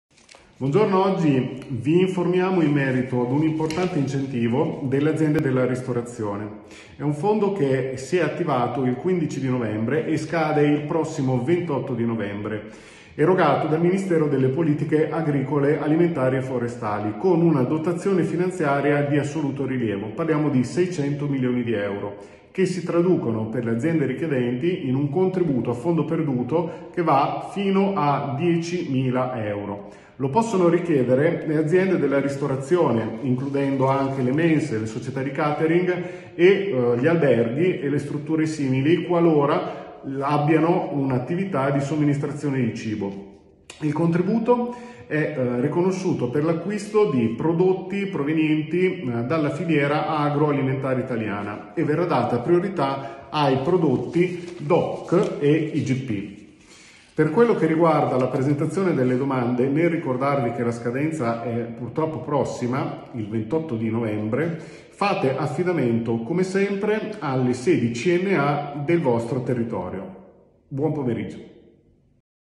Ristorazione, arrivano contributi a fondo perduto
Vanno da 1.000 a 10.000 euro i contributi a fondo perduto per le imprese della ristorazione che, dal 14 agosto 2020, hanno acquistato prodotti IGP e DOP delle filiere italiane.

In questo video Nicola Tarini, consulente di Finimpresa srl, la società del credito di CNA, spiega le principi caratteristiche dell'iniziativa.

Per la compilazione delle domande, da presentare entro il 28 novembre, ci si può rivolgere a tutte le sedi di CNA Modena